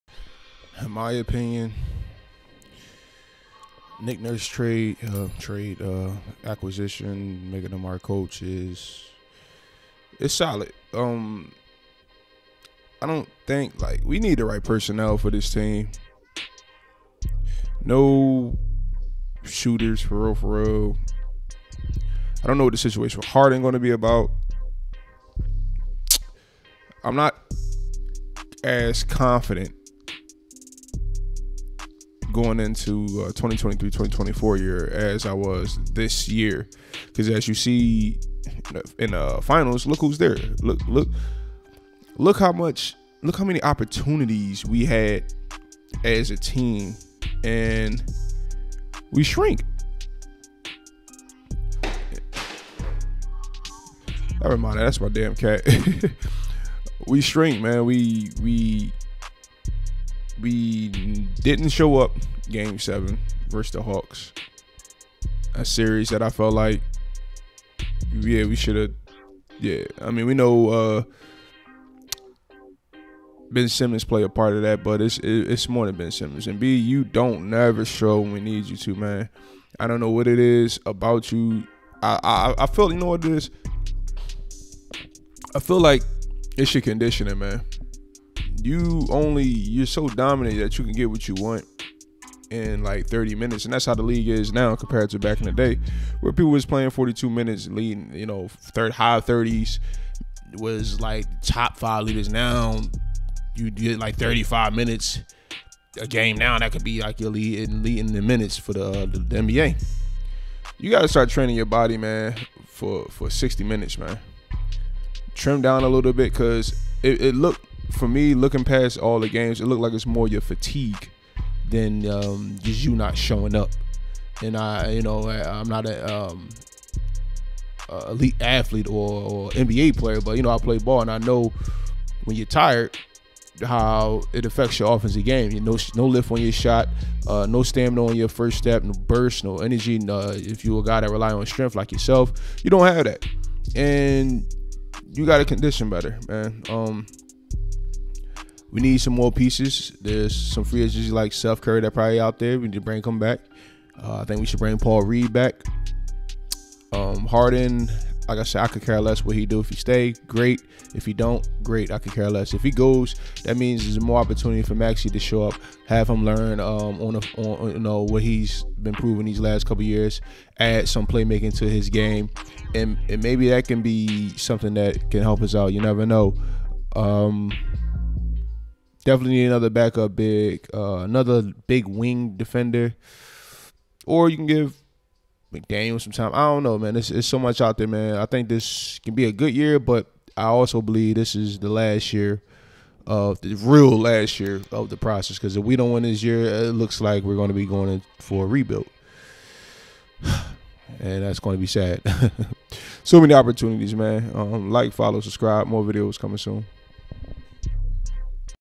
Sixers will start a rebuild after next season?!
Next season will be the last season of the sixers before a rebuild